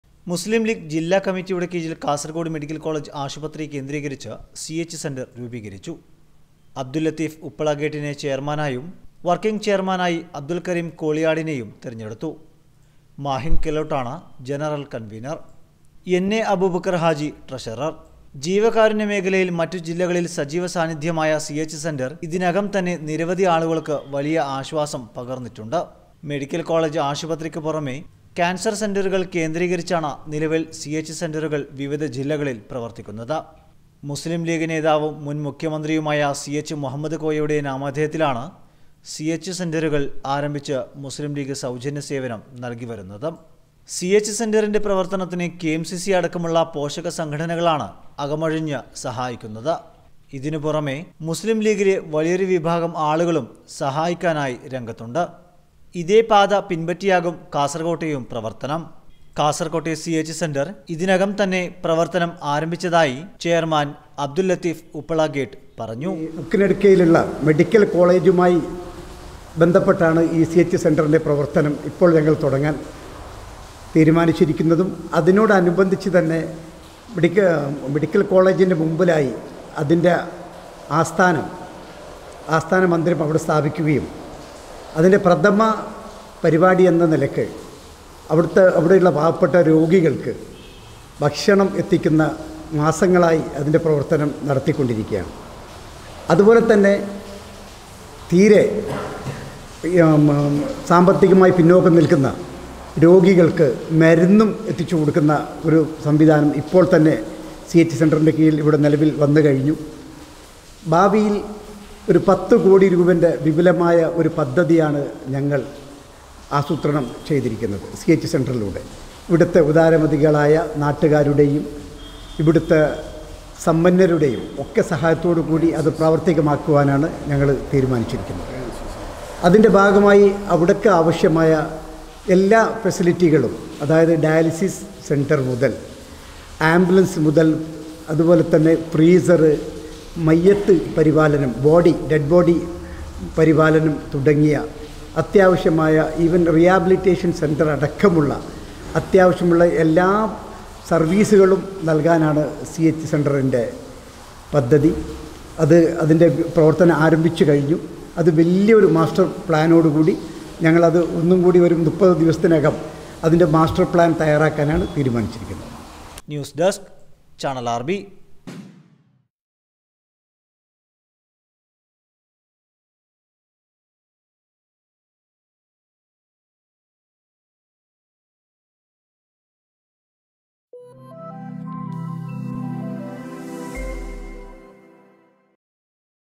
കാസർകോട് സി.എച്ച് സെൻ്ററിന് 10 കോടി | CH CENTRE KASARAGOD
മുസ്ലിം ലീഗ് ജില്ലാ കമ്മിറ്റിയുടെ കീഴിൽ കാസർകോട് മെഡിക്കൽ കോളേജ് ആശുപത്രി കേന്ദ്രീകരിച്ച് സി.എച്ച്. സെൻ്റർ രൂപീകരിച്ചു.